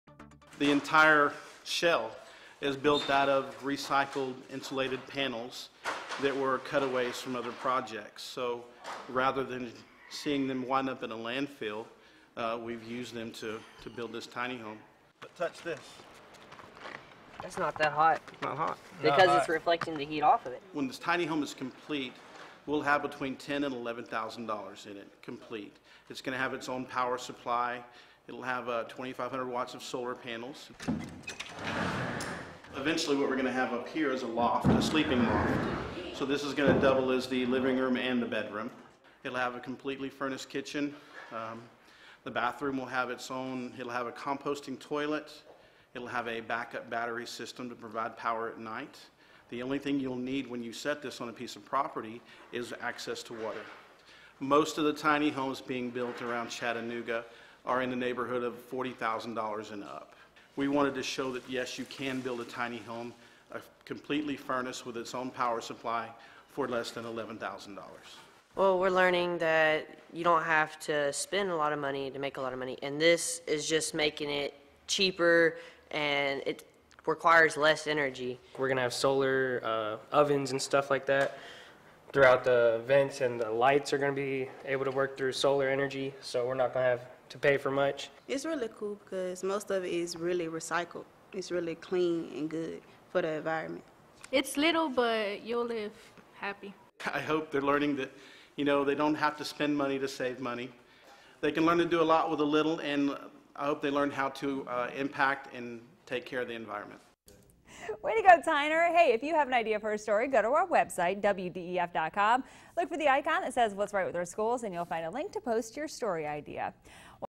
What's Right With Our Schools
Tyner students build tiny houses